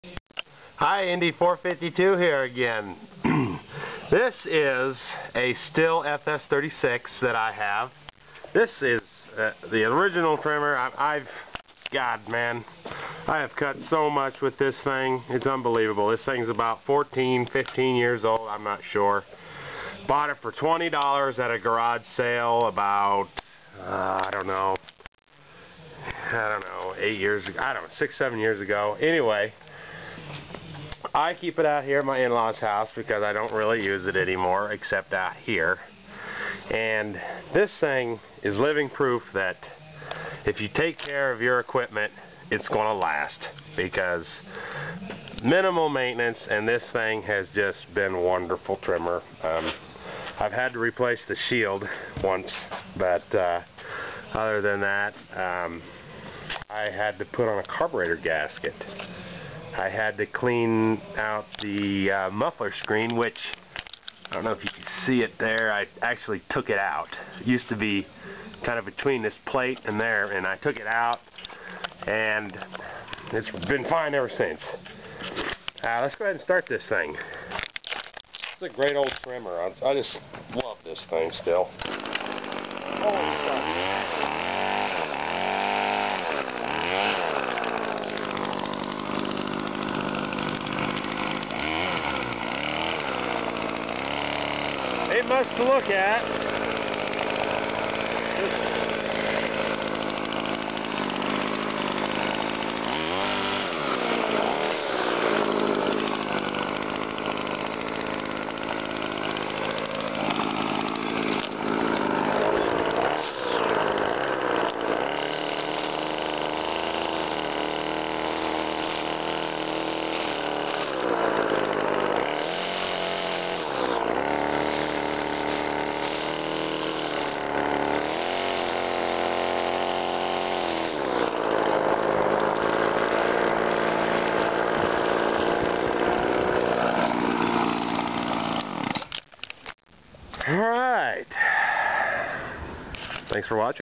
Stihl FS 36 trimmer
Heres my older Stihl FS36 trimmer demo. This has been a good machine for many years.  Its hard to hold a trimmer and camera while trying to use both!! LOL!